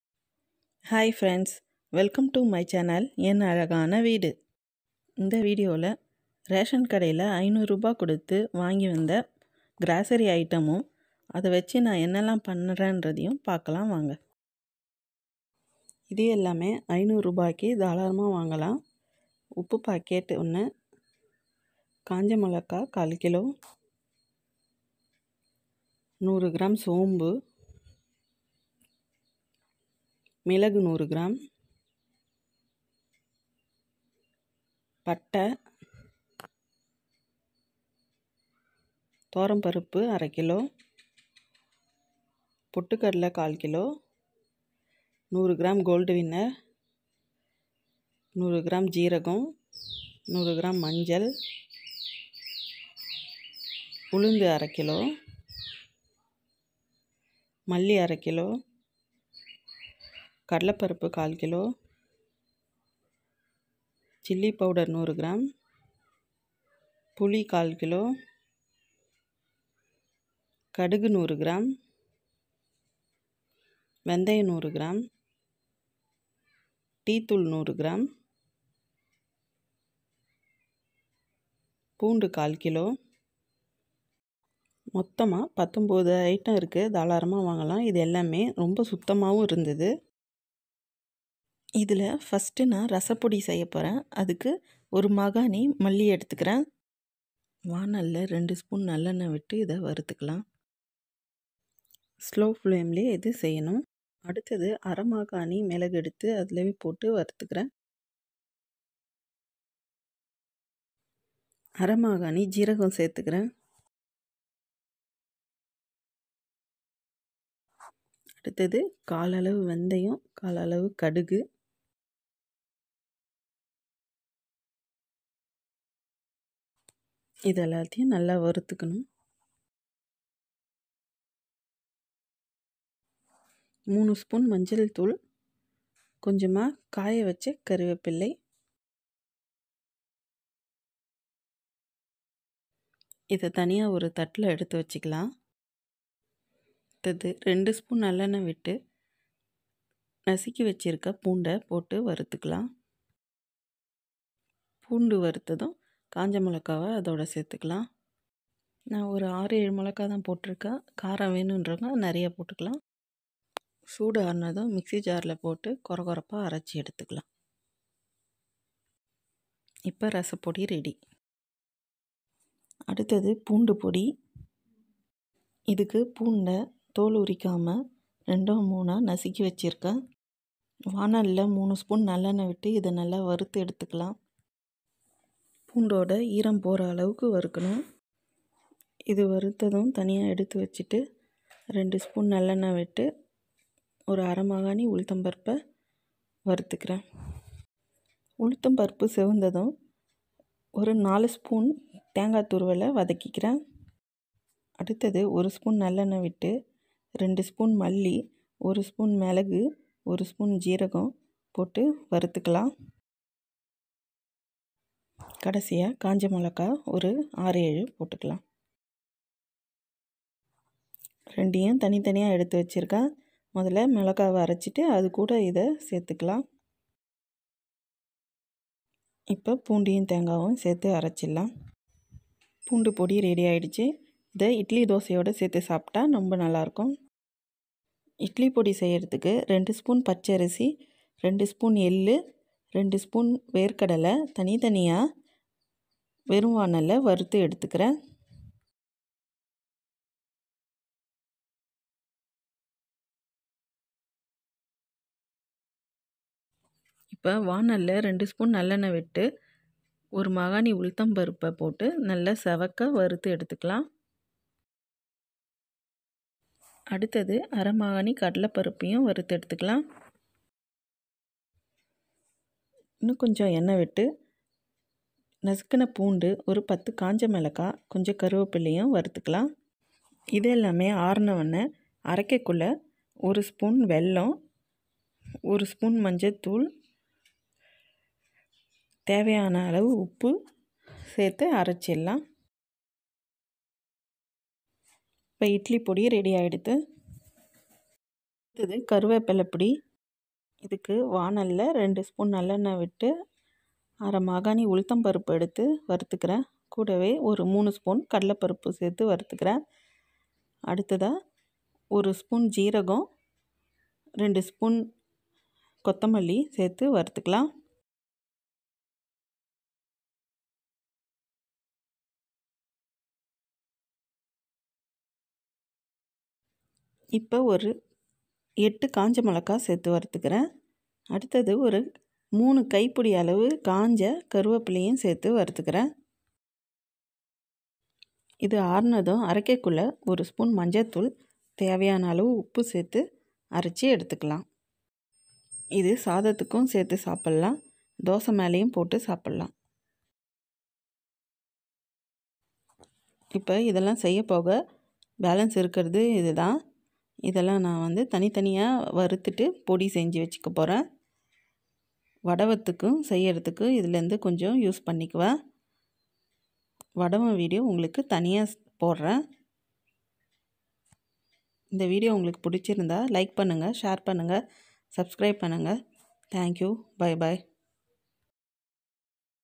Rasam podi/poondu podi/idli podi/kariveppilai podi/19 groceries item for rs. 500 from ration shop
Ration shop 19 groceries item for rs 500 review.